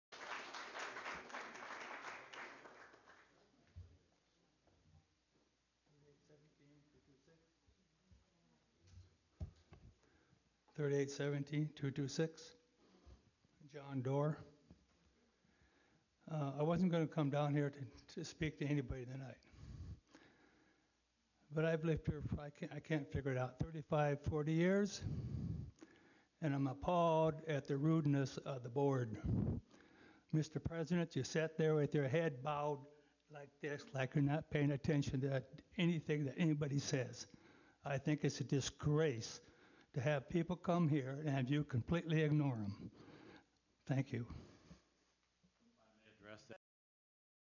Canyon Lake POA Homeowner scolds David Eilers for his rudeness
David Eilers rudeness to Homeowners is legendary. Finally, a 40 year Canyon Lake Homeowner tells him off for the other Homeowners. The Eilers Board has sued more Homeowners than any other in the past recorded history of the Board.